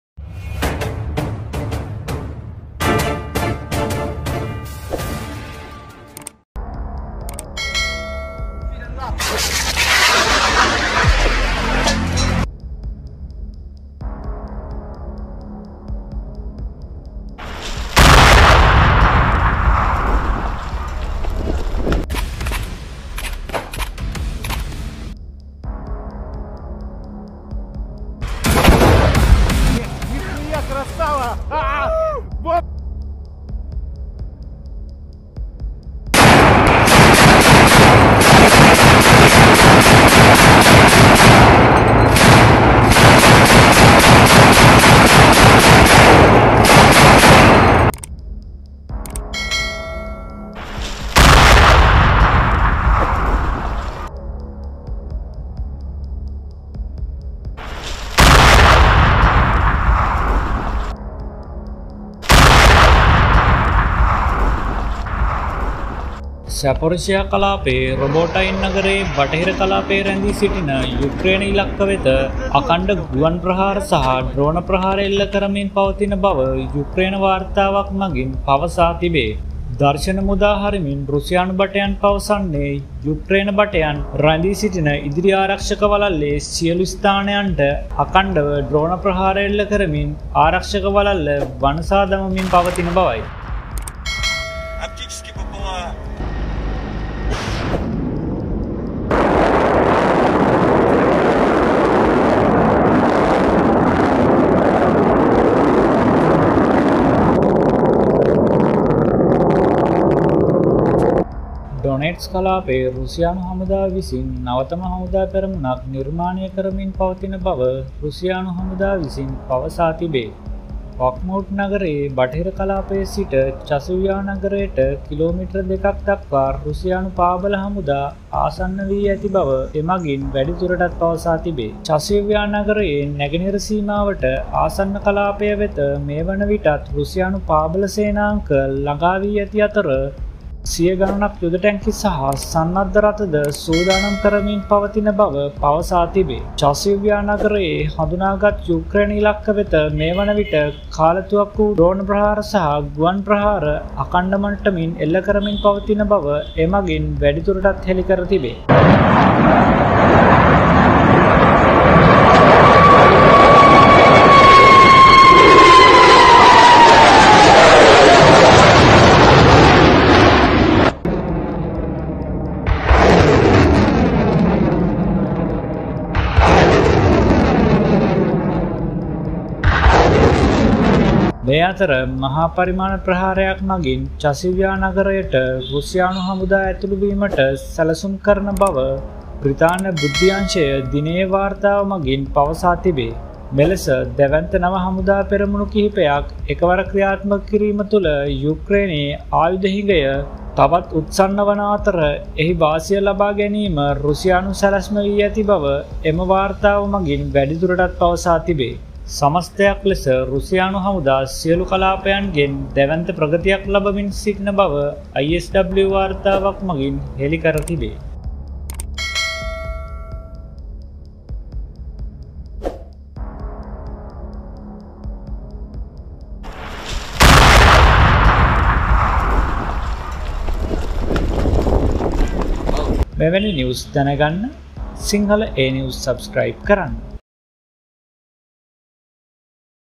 Ukraine Russia war updates Sinhala |චසීව්යා වෙත දැවැන්ත ප්‍රහාර සැලැස්මක්| Robotyne attack plan
Russian War Today updates 2024/02/26
In this video, we will discuss Ukraine being supported by more nations like USA, UK, France, Poland, and Sweden. While Russia has vetoed a resolution that was moved against Russia for entering Ukraine. NATO is also coming under pressure from the US, Germany, and the UK to take a stand against Russia. We will explain these with other latest news of the Russia-Ukraine conflict.